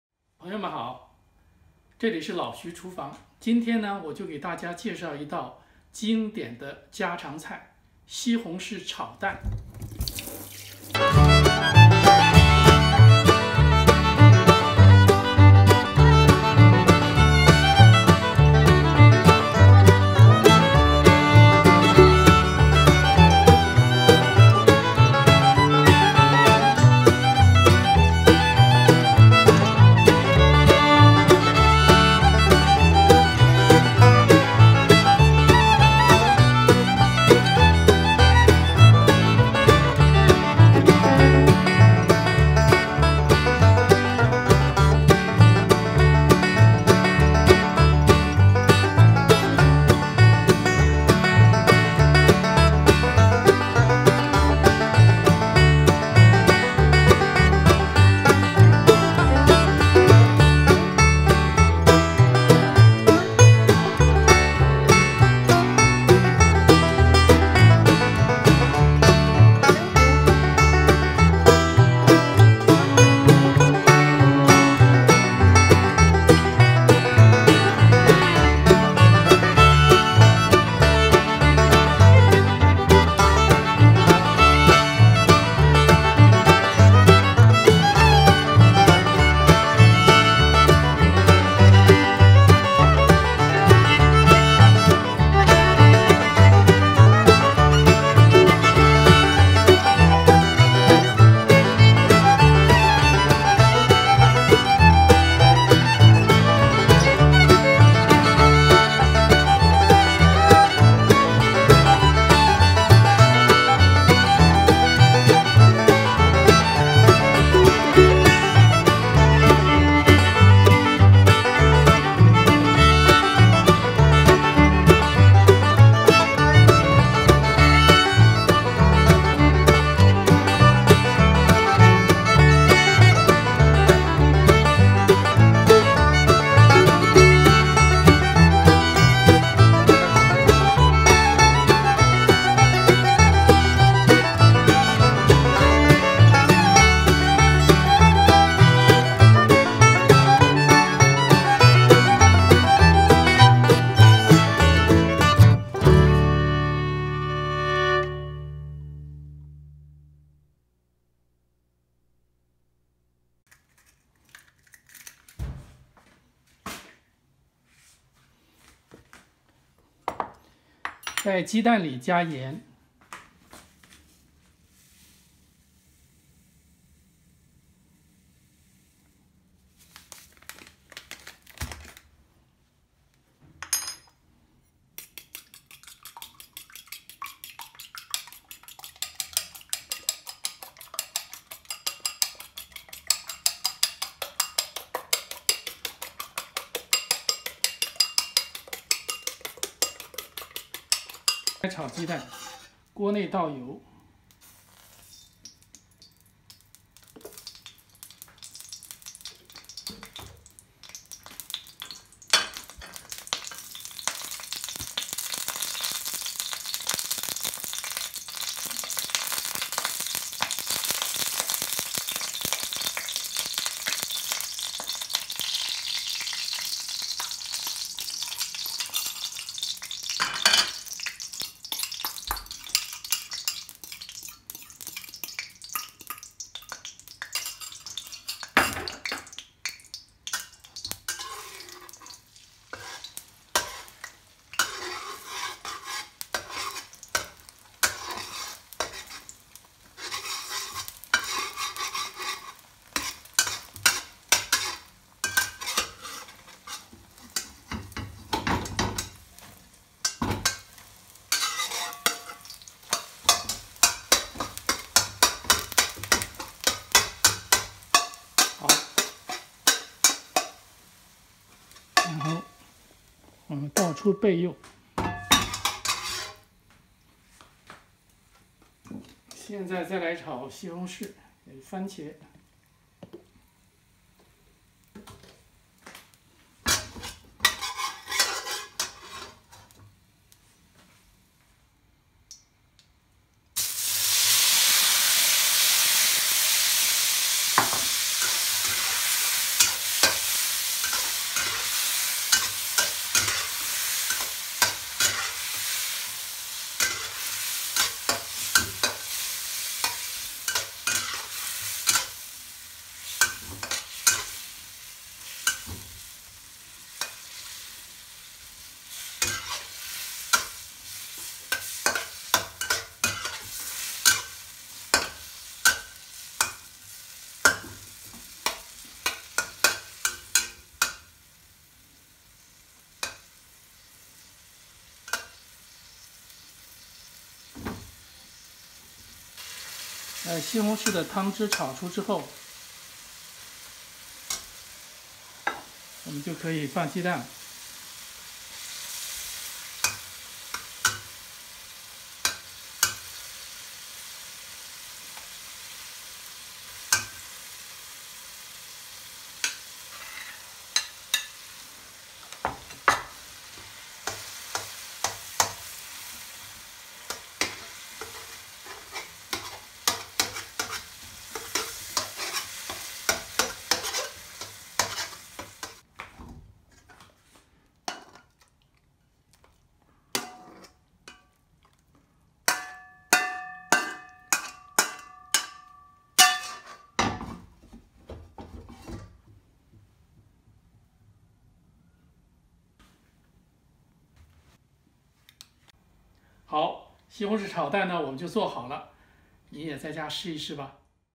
西红柿炒鸡蛋 How to Make Stir-Fried Tomato and Scrambled Eggs
家常西红柿炒蛋的做法 3 Easy steps to  to Make Stir-Fried Tomato and Scrambled Eggs 番茄炒蛋.
西红柿切小块，
打鸡蛋散，
国内放油把鸡蛋炒好，
炒西红柿，超出汤汁，
把炒好的鸡蛋放入国内一起炒2分钟
Cut the tomatoes into small pieces,
Beat the eggs scattered,
The domestic oil is used to fry the eggs.
Fried tomatoes, beyond the soup,
Put the scrambled eggs into the domestic and fry for 2 minutes